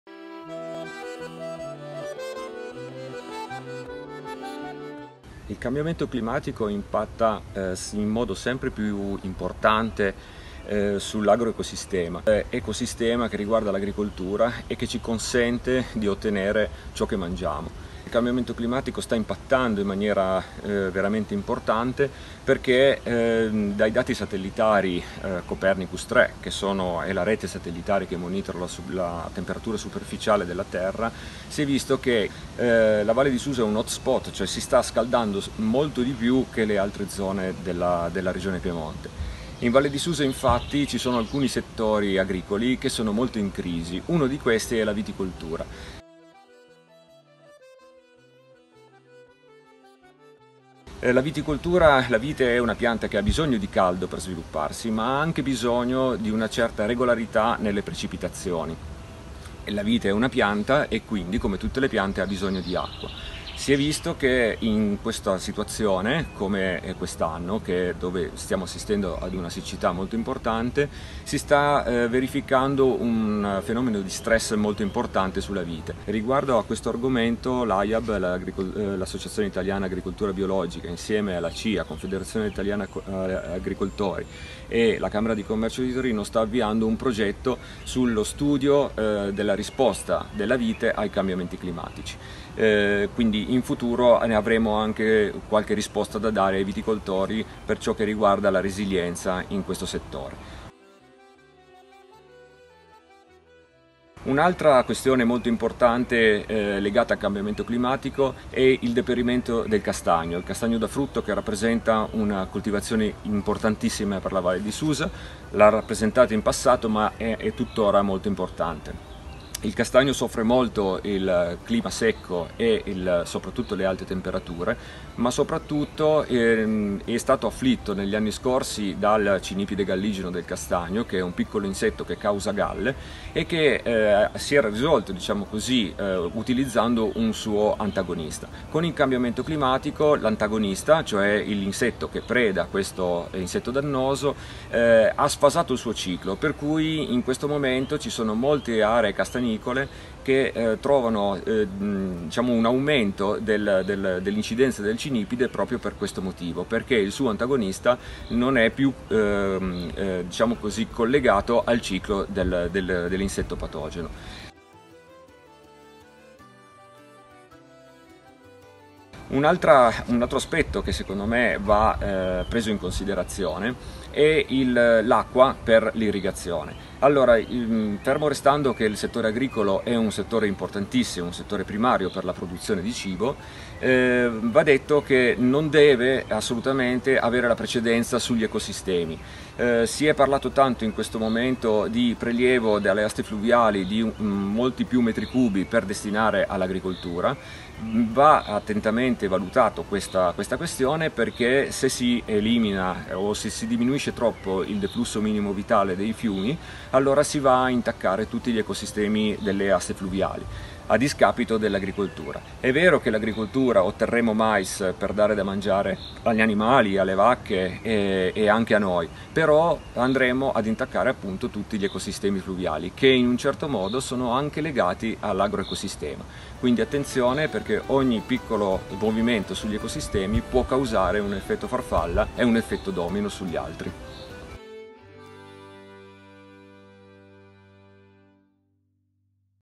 Gli effetti del cambiamento climatico in Valle di Susa -Parte 1- Intervista a Luca Cavallo, agronomo
In questo primo video affrontiamo insieme a Luca Cavallo, agronomo valsusino, alcune delle problematiche che il cambiamento climatico sta creando sul nostro territorio.